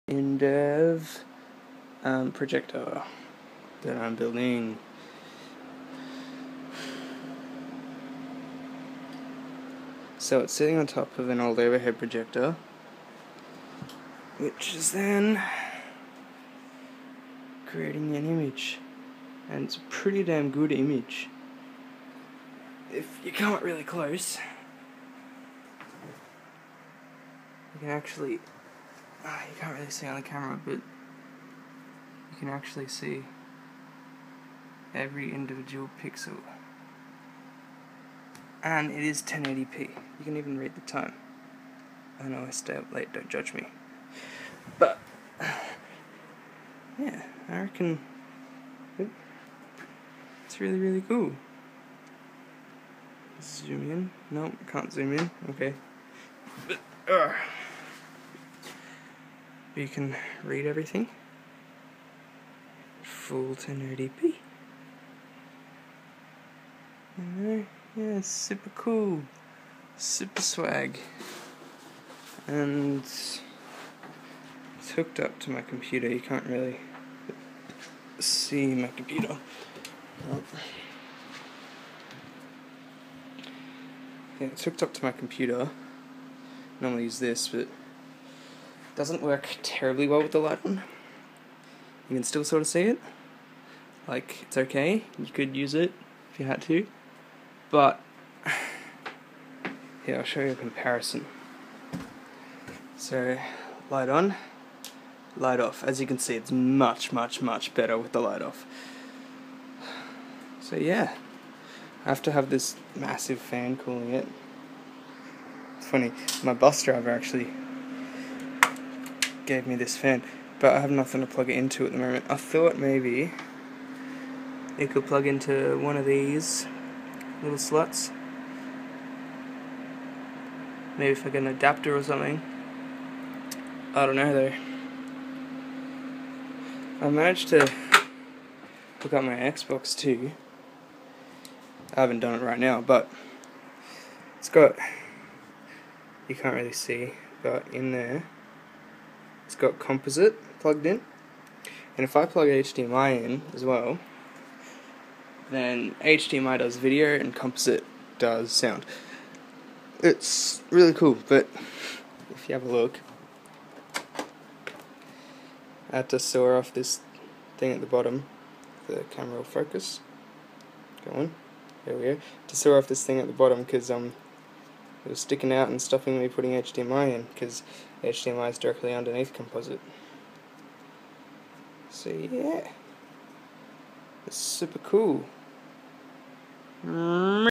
Homemade LCD projector life hack
I bought a 7" LCD monitor off eBay and picked up an overhead projector from a guy (also from eBay).
It cost me $185 overall (which is pretty good) and it's full 1080p (not 4k sadly).
After a lot of tinkering with the brightness, because the LCD turned out to be opaque rather than transparent, I finally got it to work! Lots of waiting, trial and error and hard work got me this 76.8" image on the wall! :D work = reward